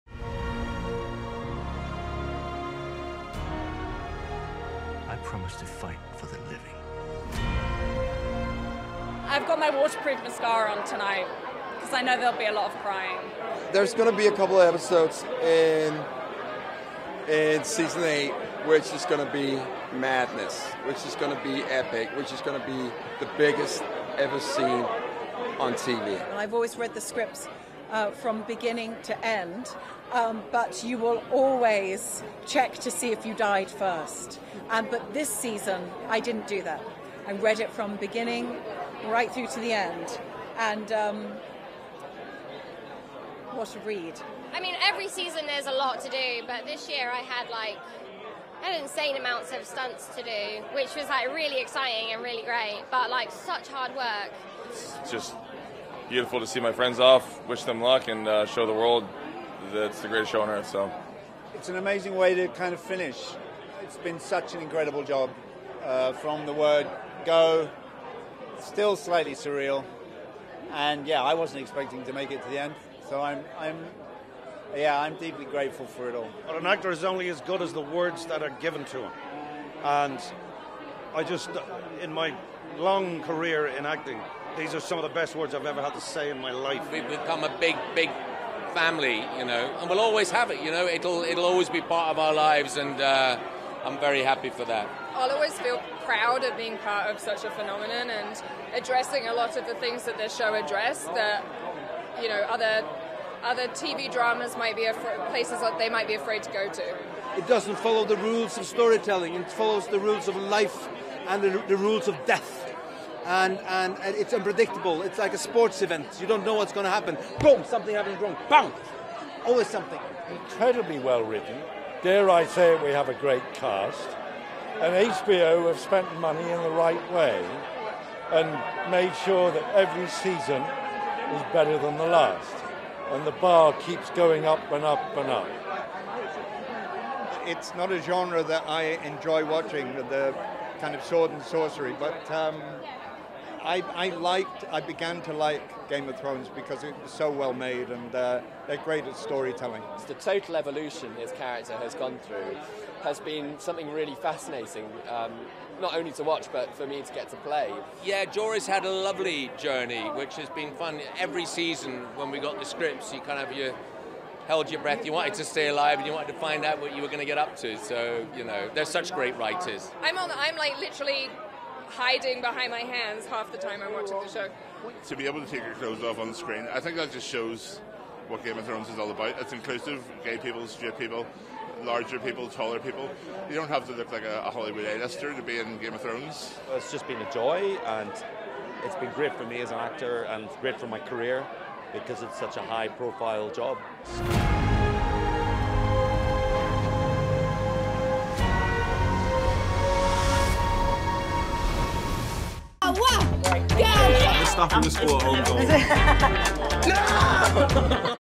Game of Thrones cast get emotional at premiere
The cast of the hit series Game of Thrones have attended an emotional premiere of the show's final season in New York. The show will air April 14th. Report by Louis Etemadi.